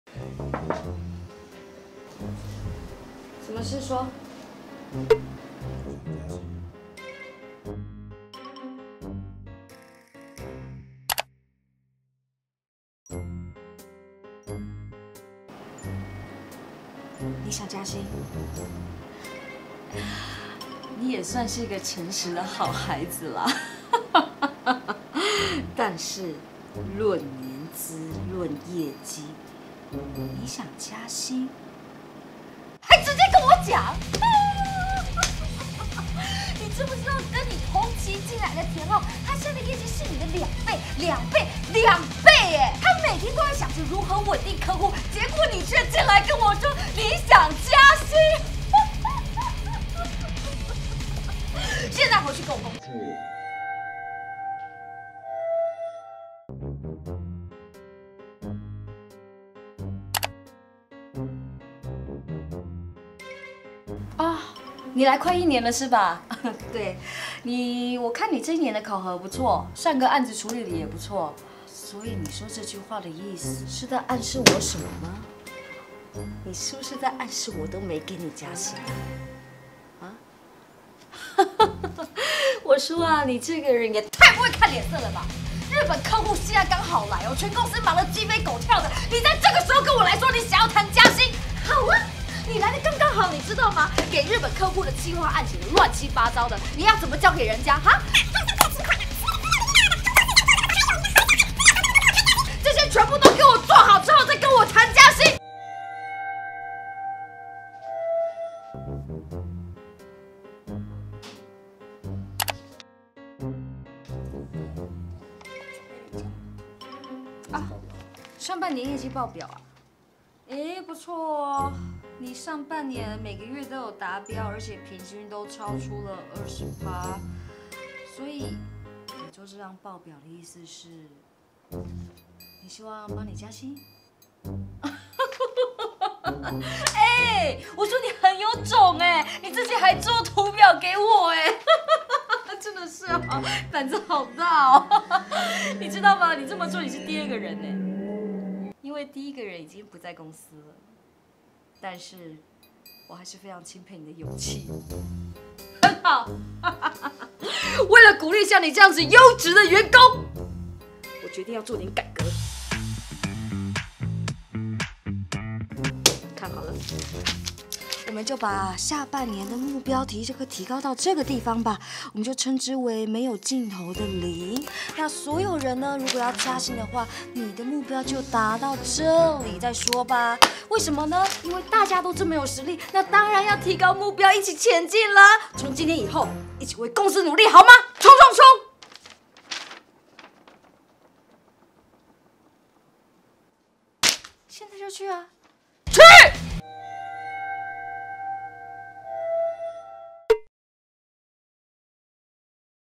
【人生選擇題Ep.08】- 如何讓老闆幫我加薪
關於提加薪這件事，
大家是不是在腦中都想過了一遍這樣的結果？
跪求臉書大神指示到底該怎麼提！

官方LINE「@fnx3248L」